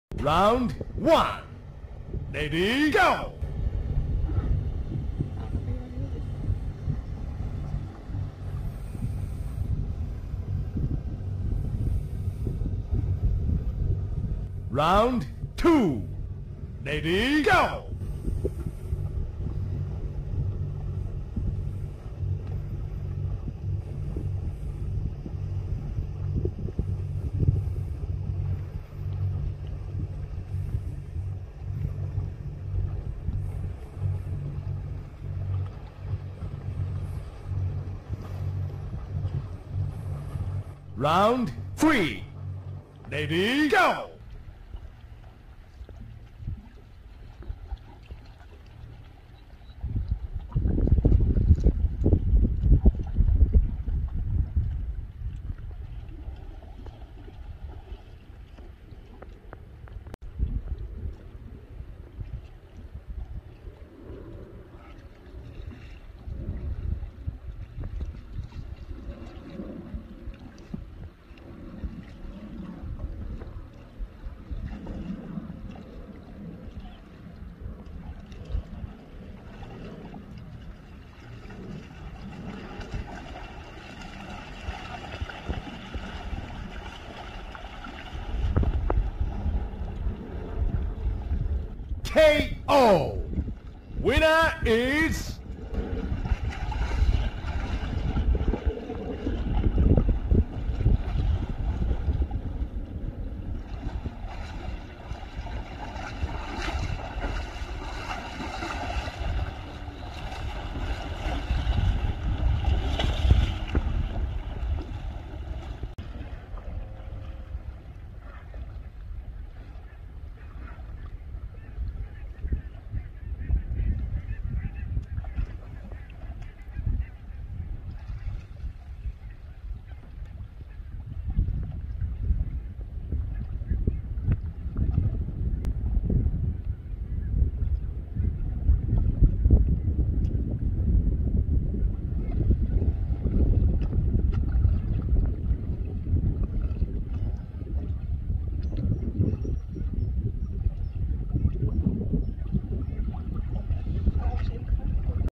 Schotse hooglanders hebben er zin in!
Schotse hooglanders bij het Lauwersmeer (Friesland) hebben er nogal zin in.

Tijdens het varen over het Lauwersmeer in Friesland kwam ik deze Schotse Hooglanders tegen. Het eerste wat mij opviel was dat ze nogal hitsig waren. Het leek mij een goed moment om vanaf een gepaste afstand dit hilarische tafereel vast te leggen.